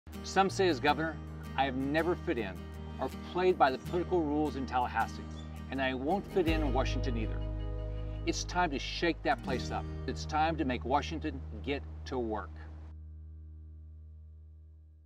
Rick Scott for Florida
Rick Scott is running to beat Bill Nelson and be the next Senator from the great state of Florida. Learn more about Rick Scott and his campaign for the US Senate